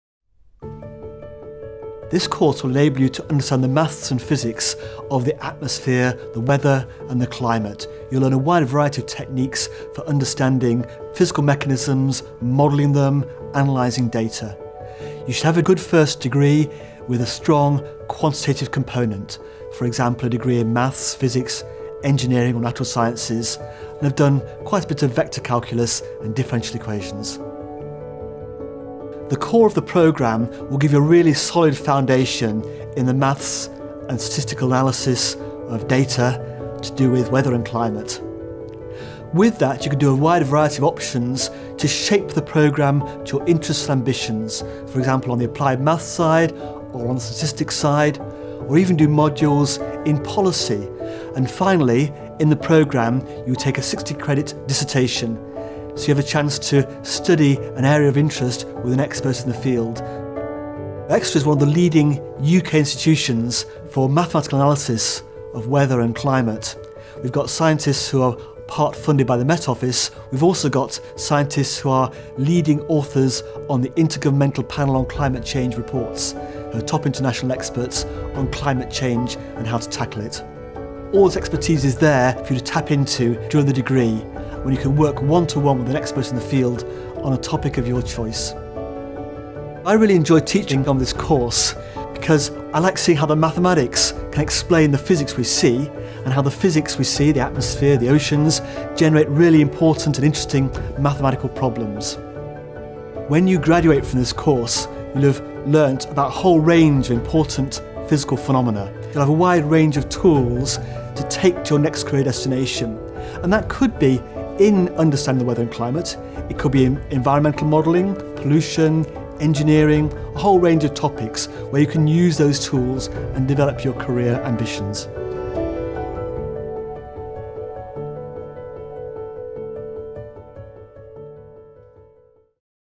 MSc Weather and Climate Science
Prof. Andrew Gilbert talks about MSc Weather and Climate Science at the University of Exeter.